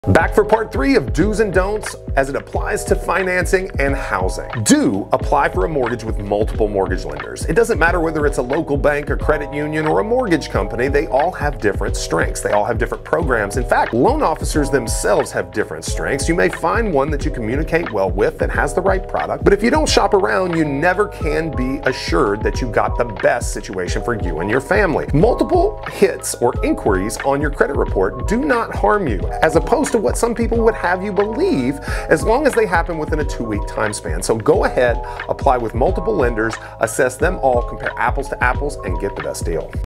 Talk to multiple mortgage lenders
Banks, credit unions and mortgage companies all have different strengths and programs. Finding a lender that you communicate well with and who has the right product is important so shop around until you find someone who’s right for you.